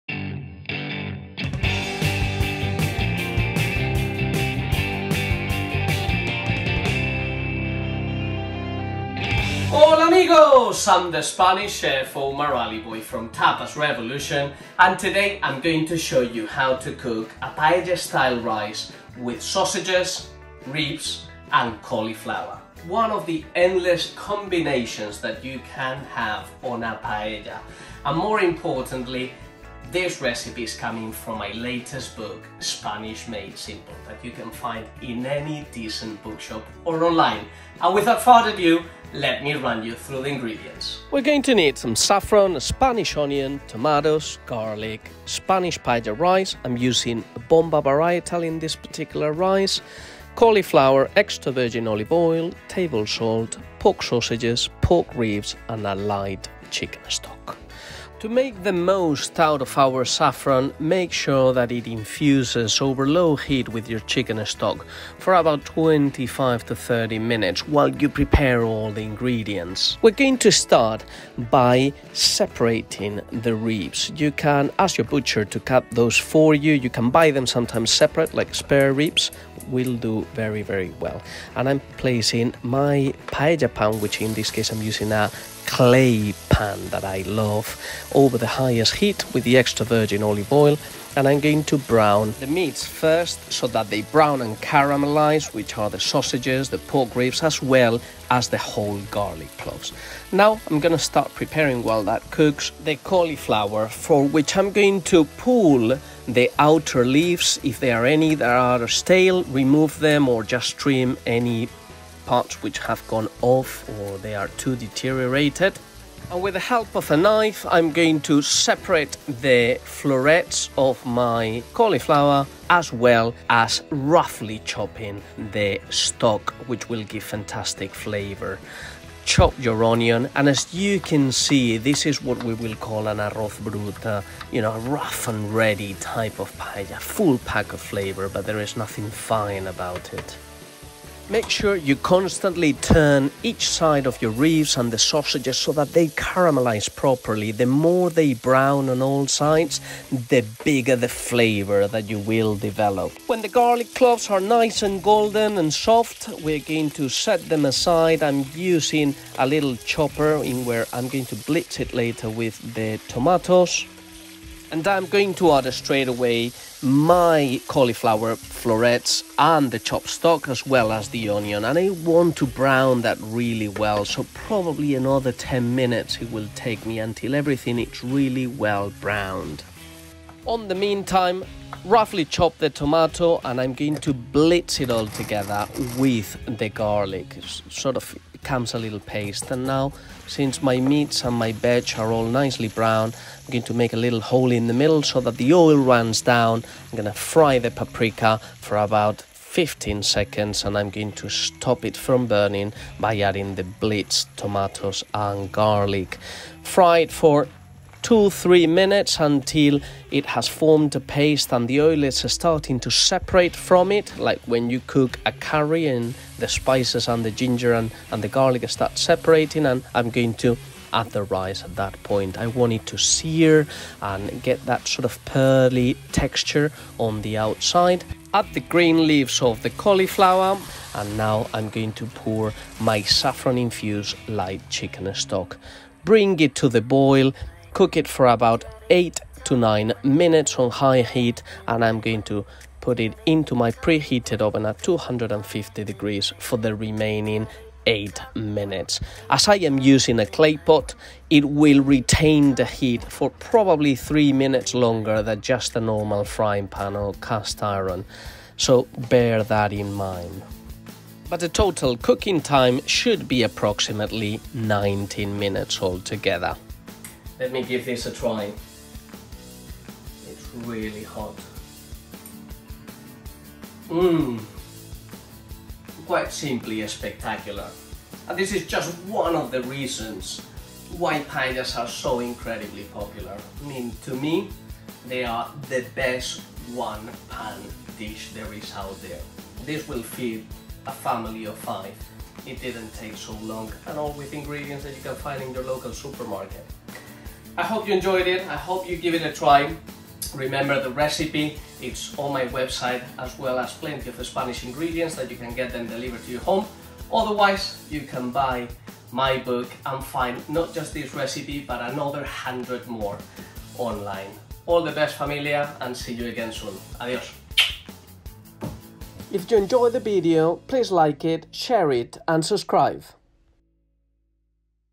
Pork ribs, sausages and cauliflower Paella
Valencia, the Mediterranean capital, is famous for its rice growing crops thanks to the humid weather and the wetlands of the Albufera Natural Park . Everyone in this part of Spain knows how to cook their rice in many different ways and this recipe is one of them. They like saying that they have a different rice recipe for each day of the month which is actually truth. This is a quick and easy “dirty” type of rice which is full of flavour and using everyday ingredients.

Gracias amigos,